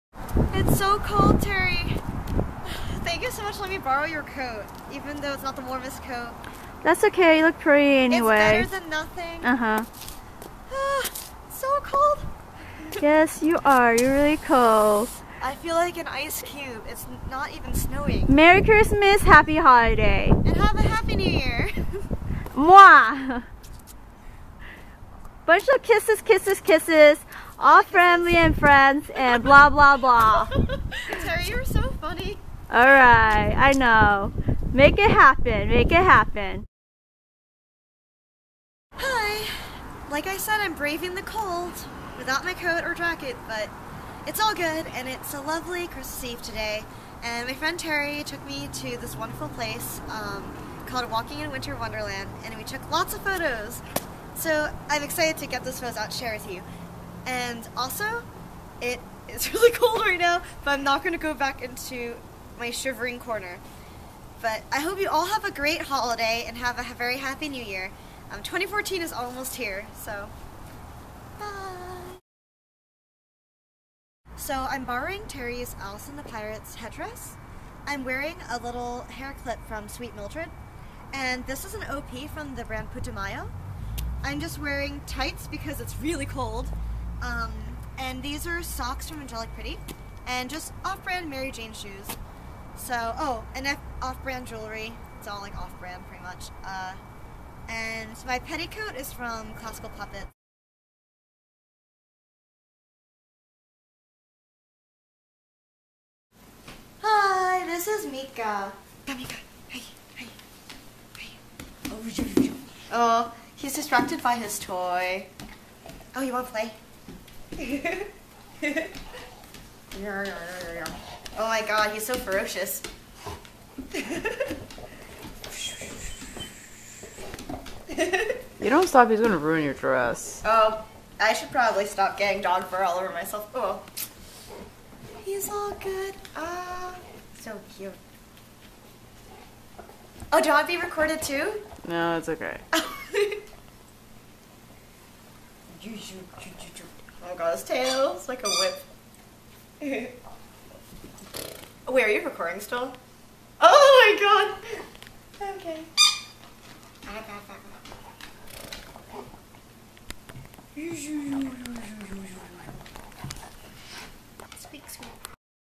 Christmas Eve in Lolita
Yesterday, my friend Terry (who I call my lolita big sister) came to pick me up for some winter festivities. This was the first time I actually did something on Christmas Eve. She took some random clips of me, and I compiled them together in this little video. Sorry about the shakiness; we didn't have a tripod!

This was all done on a whim, so it is quite silly and random.